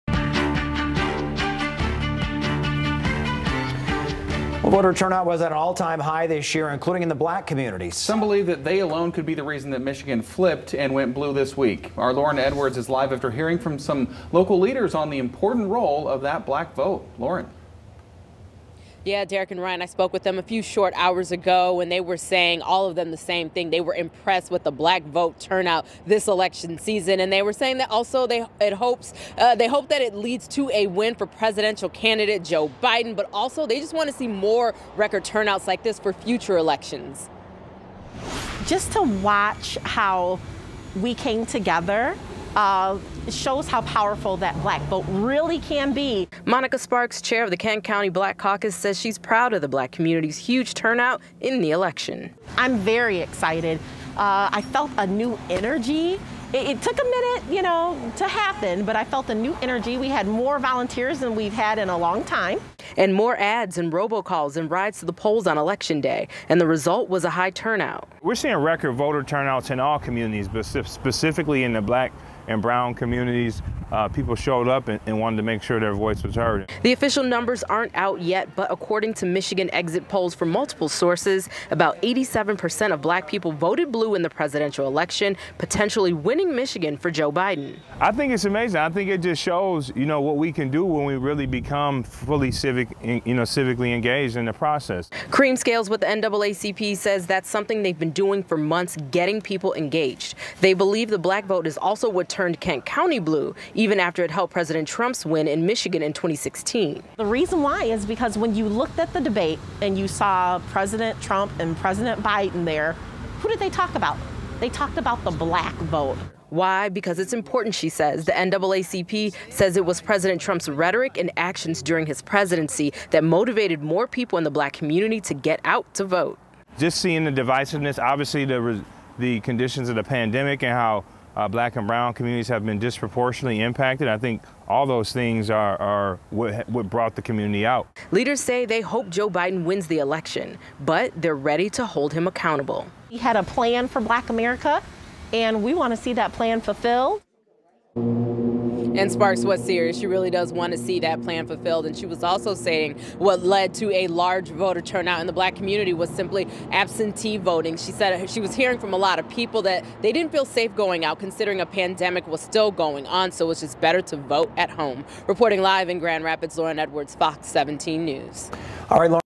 Black community sees high voter turnout; local official says “I felt a new energy”
Local leaders in Grand Rapids believe high Black voter turnout helped state of Michigan and Kent County turn blue.